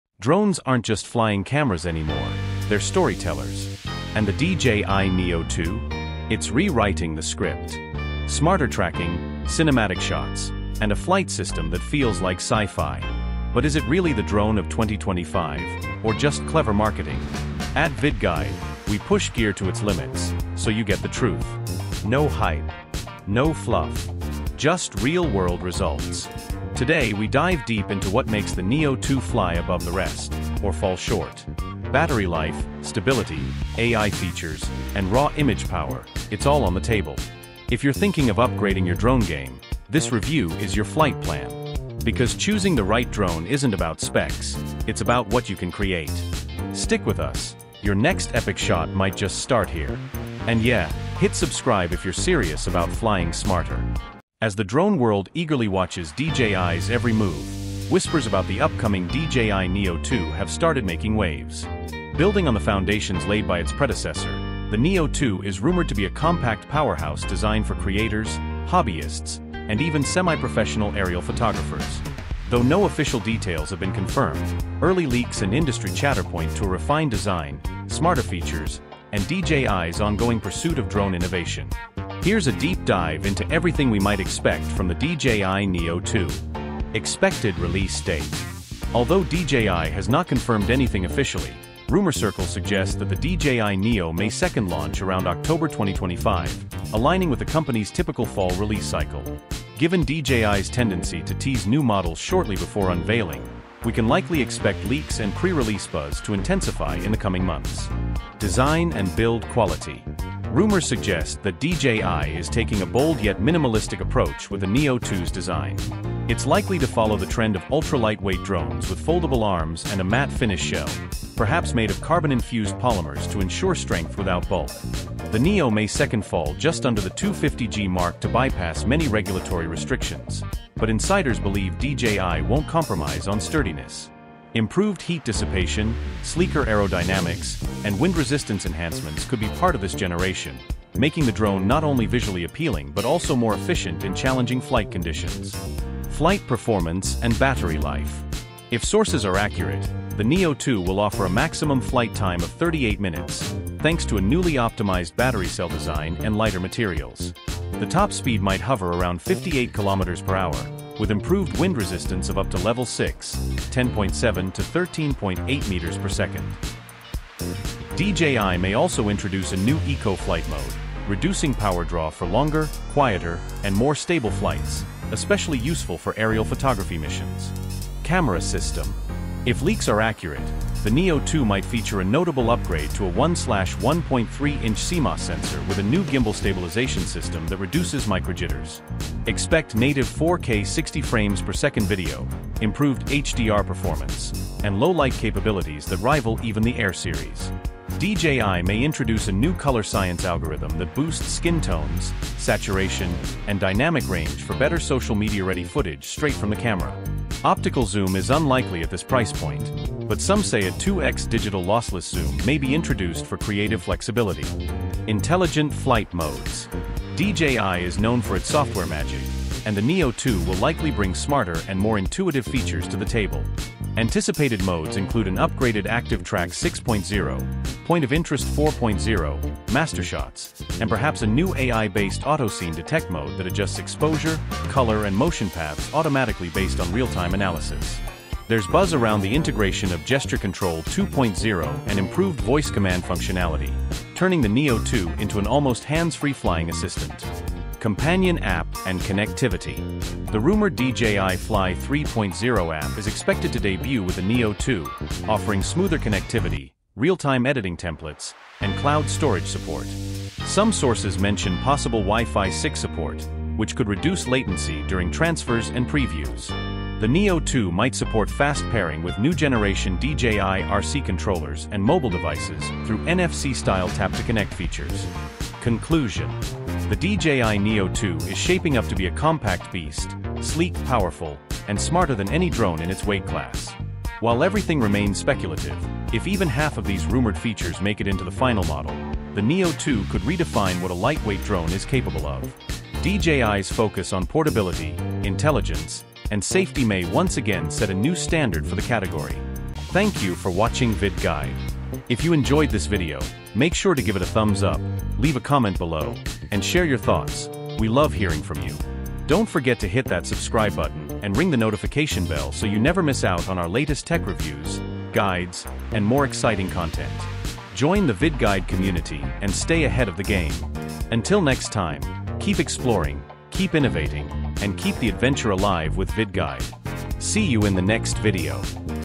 DJI Neo 2 Officially Revealed - Full Specs, Features & Launch Price Breakdown!
The DJI Neo 2 is DJI's next leap into compact and intelligent drone technology, offering a sleek design with advanced features for everyday creators. With upgraded obstacle avoidance, improved flight stability, and enhanced image quality, the Neo 2 is designed to be both beginner-friendly and powerful enough for pros. At VidGuide, we break down everything you need to know about this drone—from flight time and camera specs to smart features that set it apart from the competition.

Whether you're capturing cinematic footage or just exploring aerial photography for the first time, the DJI Neo 2 brings a balance of innovation and performance that makes it a standout in its class. In this video, VidGuide takes you through the key features, potential use cases, and whether it’s worth the upgrade.